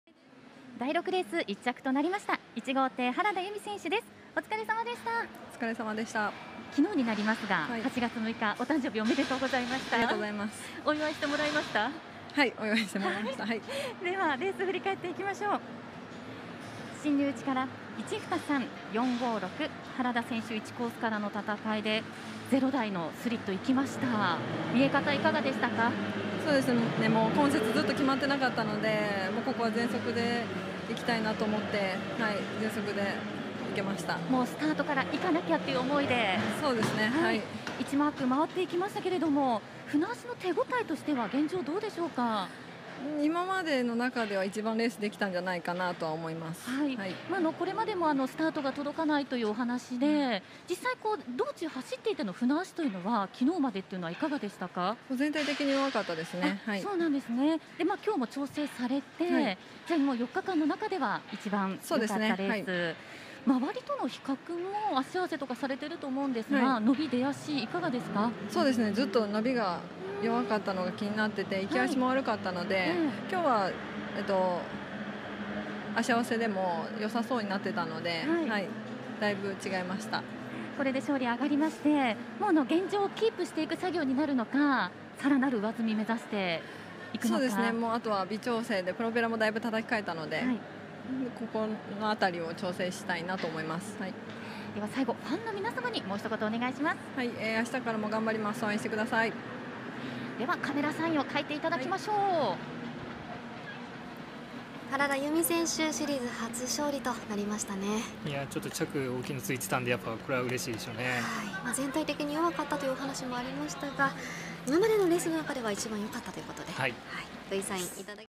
原田佑実🚤勝利者インタビュー&カメラサイン(ボートレース浜名湖・PG1レディースチャンピオン・4日目、8日7日、6R)
6R予選
勝式 組番 払戻金 人気
３連単 1－3－2 ¥1,500 4
３連複 1＝2＝3 ¥370 1
２連単 1－3 ¥660 3
２連複 1＝3 ¥510 3
拡連複 1＝3 ¥210 5
1＝2 ¥190 2
2＝3 ¥150 1
単勝 1 ¥300
複勝 1 ¥140
3 ¥140
着 枠 レーサー レースタイム
１ 1 4372原田　　佑実 1'51"4
２ 3 4050田口　　節子 1'53"3
３ 2 4190長嶋　　万記 1'54"3
４ 4 3518倉田　　郁美 1'56"2
５ 6 3993永井　　聖美
６ 5 4450平高　　奈菜

(レース映像🚤競艇・水神祭・ボートレース宮島9月8日)"